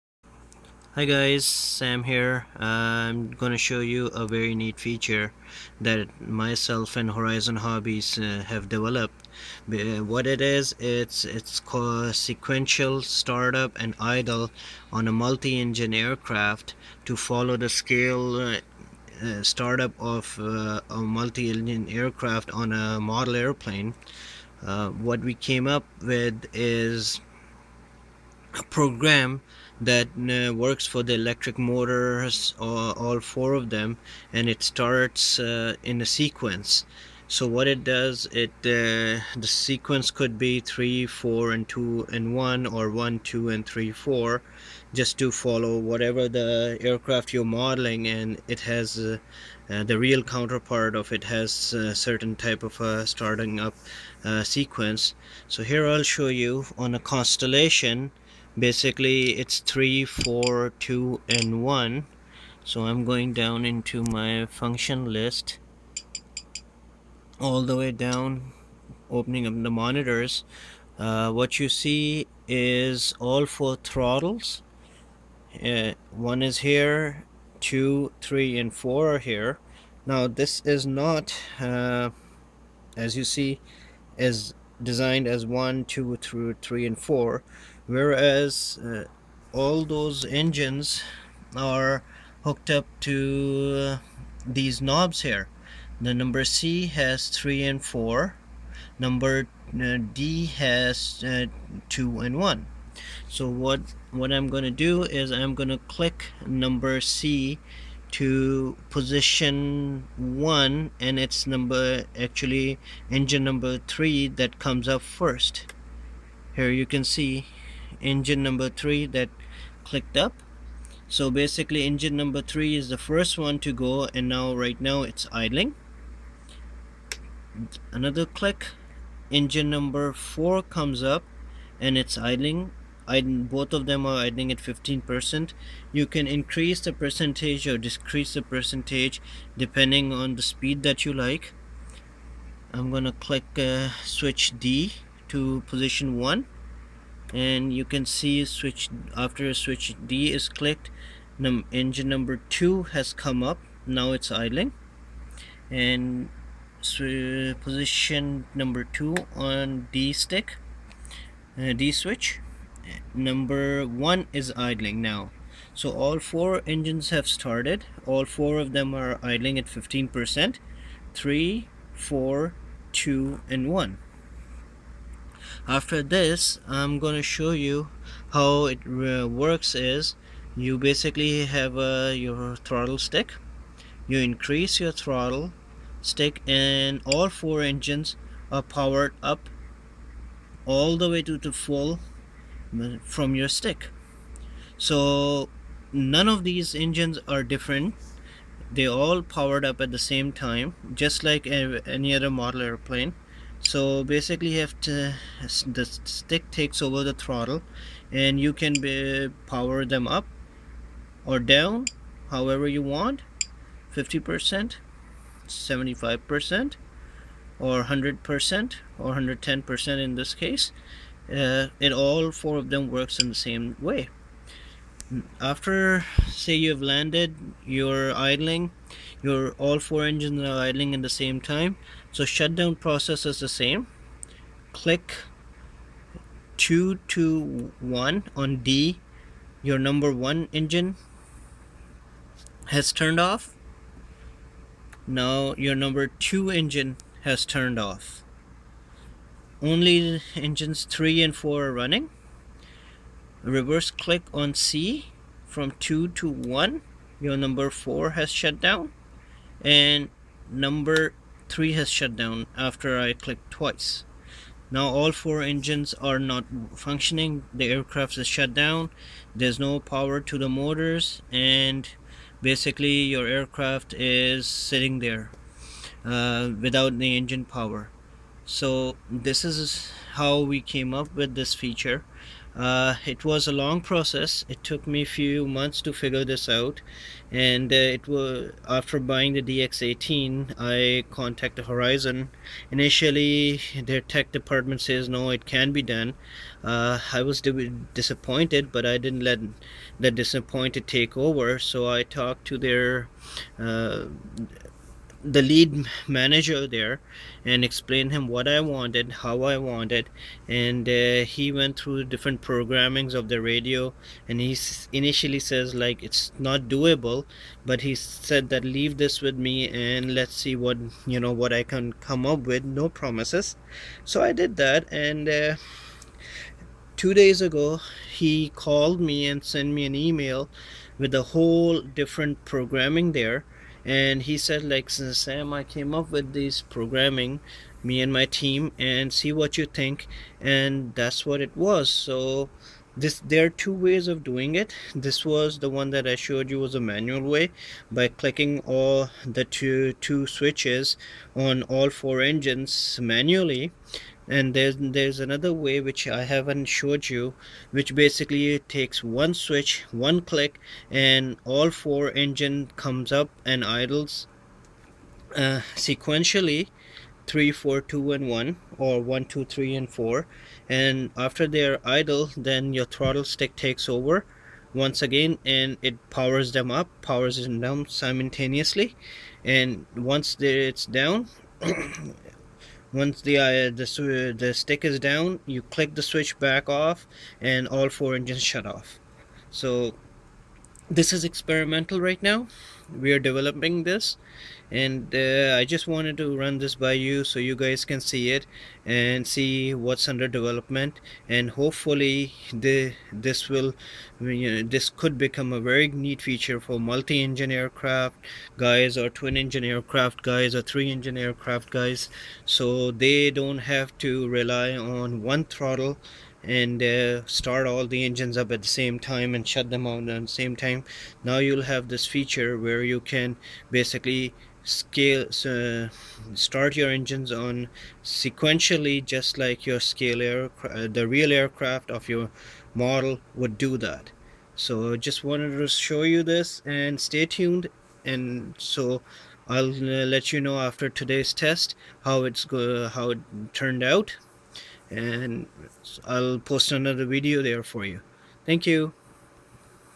Spektrum DX18 Multi engine start up and idle sequence. "Experimental stage presently"
I wanted to have a feature on Spektrum DX18 where twin engine, 3 engine and 4 engine aircraft modelers could have this opportunity to be able to start there model engines in same start up and idle sequence as real aircraft they modeled after. I came up with this idea and presented to Horizon Hobbies with my needs and requirement specifications, initially it was turned down but after a long talk with technical department's manager this has become a reality!. This is still under development and experimentation on my DX18 only for my Super Constellation model, but if all goes good, I'm sure Horizon Hobbies will launch this as a upgrade for all DX18 users who wish to use it.